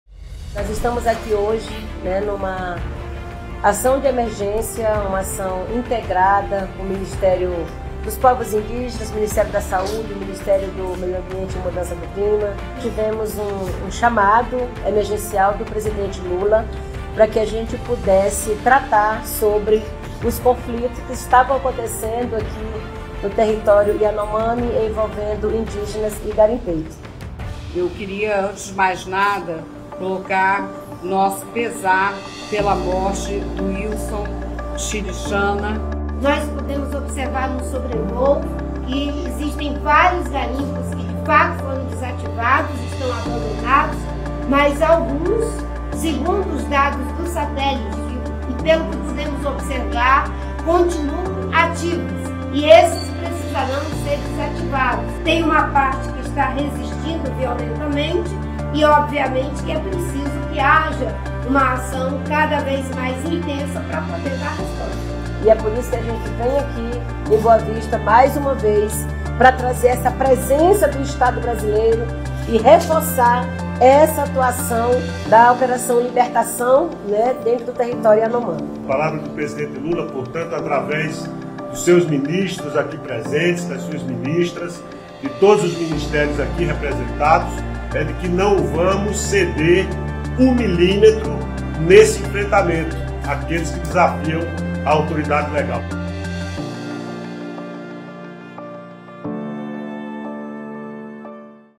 Governo está combatendo violência no território Yanomami
O governo do presidente Lula fez uma ação emergencial por meio do Ministério dos Povos Indígenas, do Ministério da Saúde e do Ministério do Clima e do Meio Ambiente, depois de três indígenas Yanomamis terem sido baleados em Roraima por garimpeiros ilegais. Ilson Xirixana, 36 anos, levou um tiro na cabeça e morreu. Ele atuava como agente de saúde.

Acabar com o garimpo ilegal é um dos compromissos do governo brasileiro. Muitos deles já foram desativados, mas parte dos garimpeiros clandestinos resiste violentamente, segundo a ministra Marina Silva.

A Operação Yanomami continuará atuando para defender os direitos dos povos indígenas e para combater tanto o garimpo ilegal como a violência no território.

🎥 Audiovisual/PR